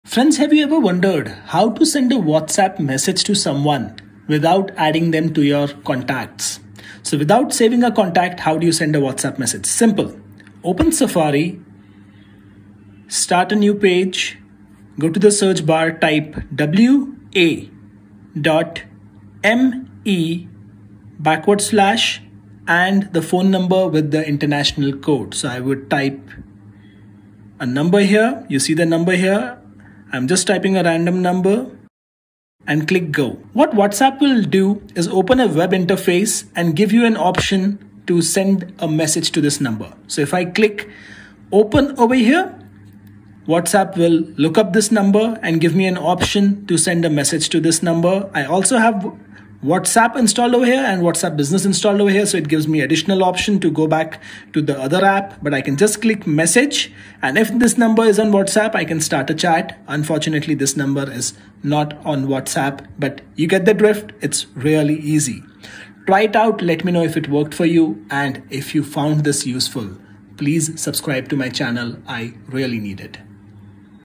How to Send WhatsApp Message on iPhone Without Saving Number?
Send WhatsApp messages without adding mobile number. You don't need to save the mobile number of the recipient on your iPhone. There is an option or a website that enables you to interact with anyone on WhatsApp without saving their number. Watch how to use it in this video.